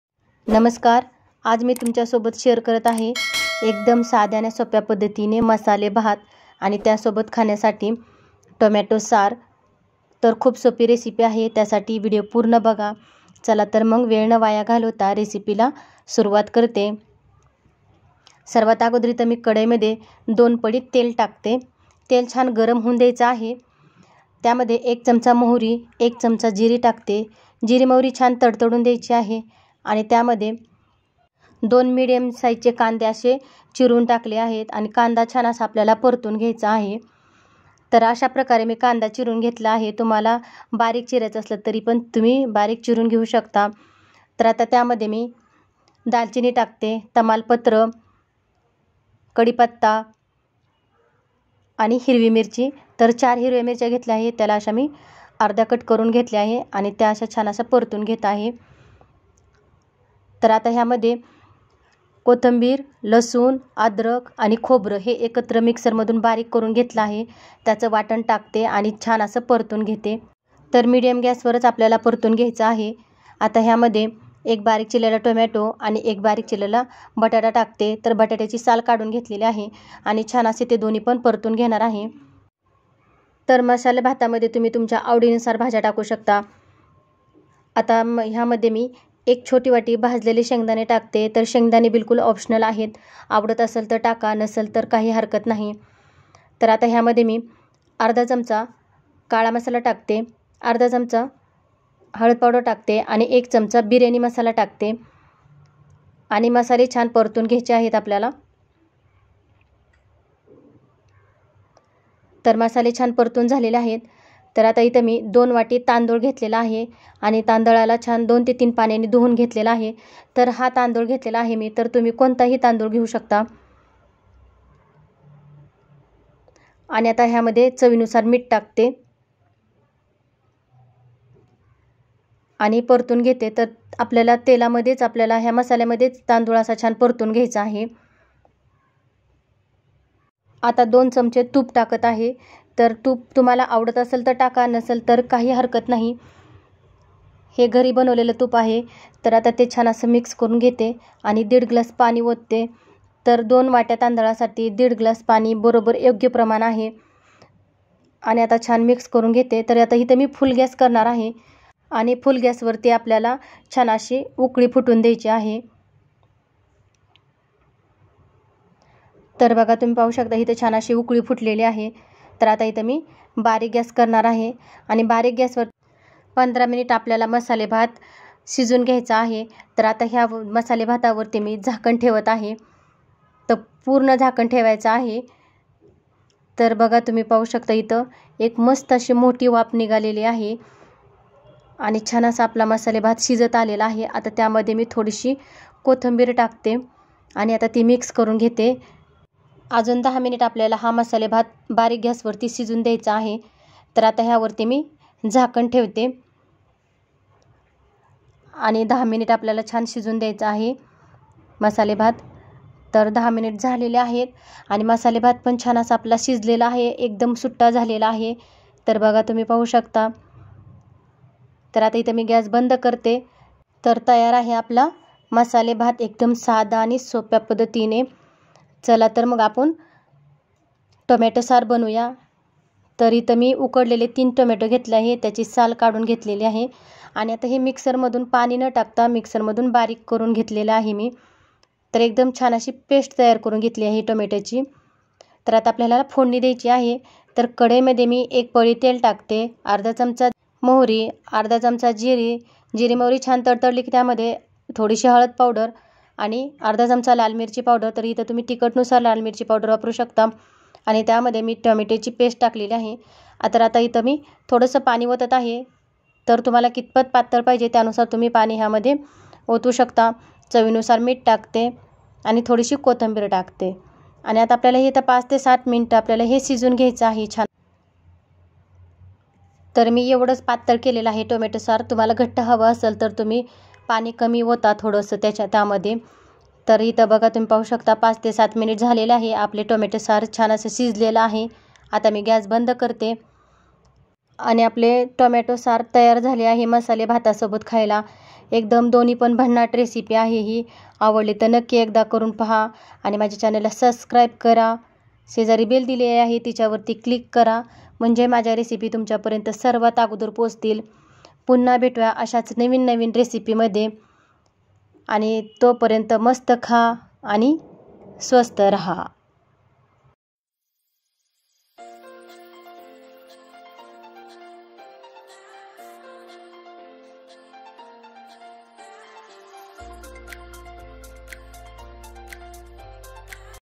खुप सोप्या पद्धतीने मासाले भात बनवला आहे आणि त्या सोबत खाण्यासाठी टोमॅटो सार
साहित्य...
दोन बारीक चिरलेला कांदा,
एक बारीक चिरलेला टोमॅटो,
कढीपत्ता,
तेल दोन पळी,
मोहरी,
जिरे,
हळद,
बटाटा एक,
भाजलेले शेंगदाणे आवश्यकतेनुसार,
पाच हिरवी मिरची,
कोथिंबीर,
लसूण+ खोबरे +कोथिंबीर +अद्रक चे वाटण,
दोन वाट्या तांदूळ,
पाणी दीड ग्लास,
मीठ चवीनुसार,
दोन चमचे तूप,

टोमॅटो सार बनवण्यासाठी लागणारे साहित्य,
तीन टोमॅटो उकडलेली,
एक पळी तेल,
जिरे अर्धा चमचा,
 मोहरी अर्धा चमचा,
हळद चिमूटभर,
लाल मिरची पावडर अर्धा चमचा ,
पाणी आवश्यकतेनुसार,
मीठ चवीनुसार,
कोथंबीर